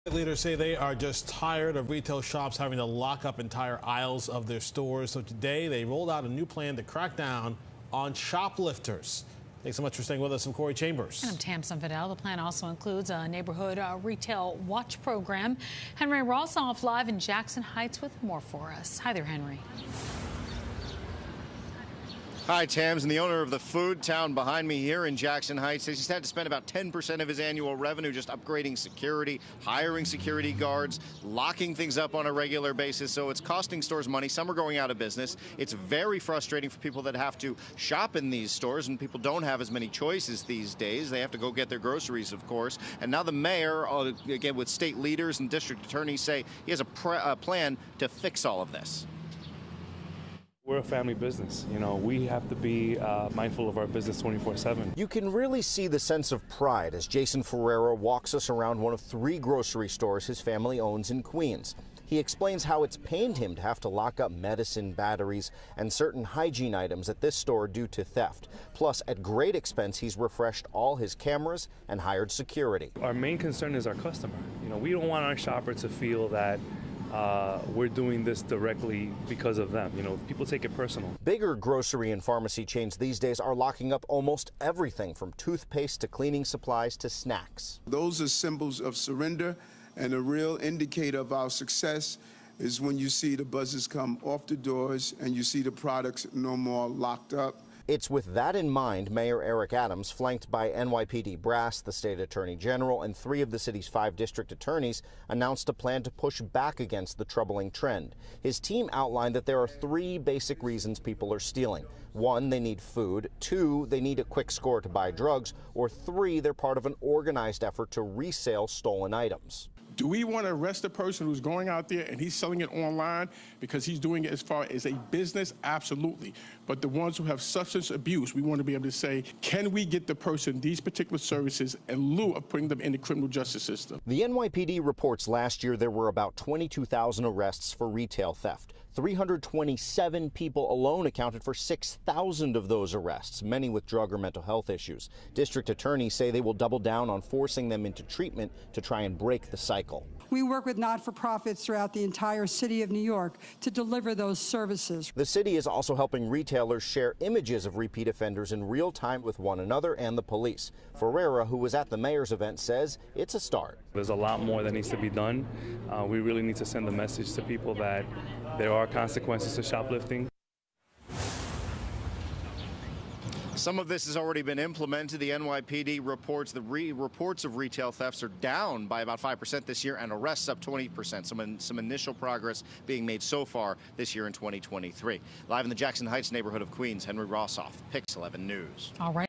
Tired of seeing everything locked up behind plastic?
City and state leaders are tired of retail shops having to lock up entire aisles of their stores.